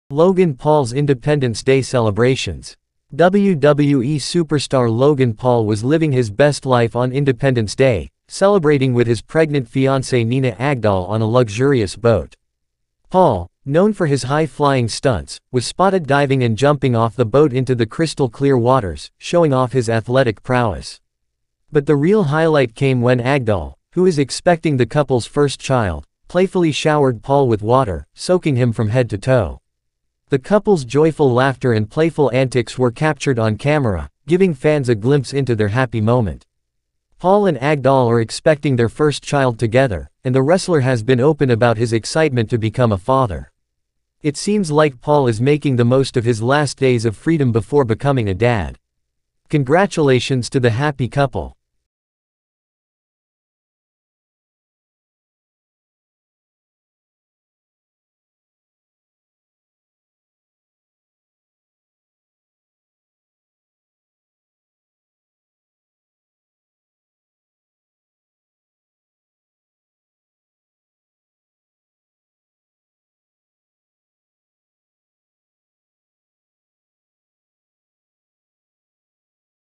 Logan Paul showered down by pregnant fiancee as he enjoys boat celebrationsWWE superstar Logan Paul
Logan Paul Celebrates 4th of July with Pregnant Fiancée Nina Agdal
. WWE's Logan Paul Enjoys Boat Festivities with Fiancée Nina Agdal
 Logan Paul and Pregnant Fiancée Celebrate Independence Day on a Boat
4. Logan Paul and Nina Agdal Dive into 4th of July Celebrations